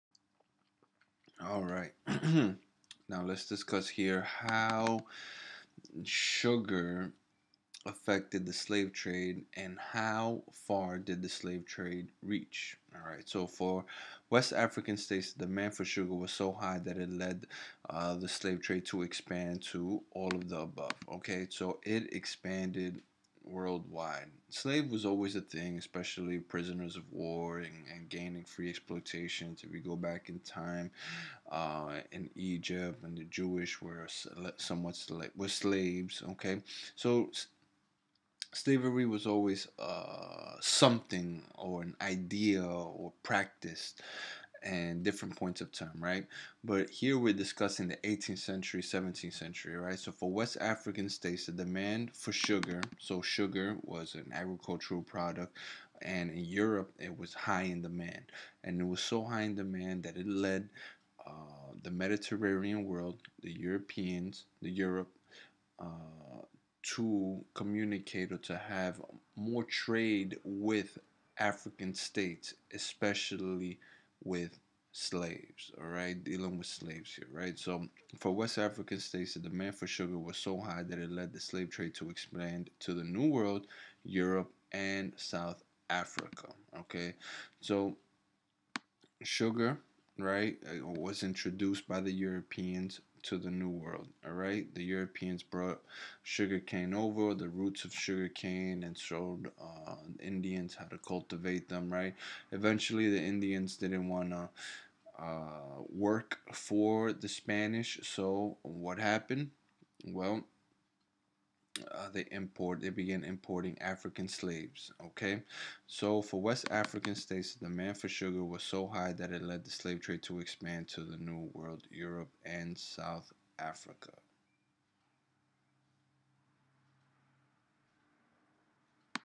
U.S. History A5 (11/20) West African states
For West African states, the demand for sugar was so high that it lead the slave trade to expand to the New World, Europe, and South Africa.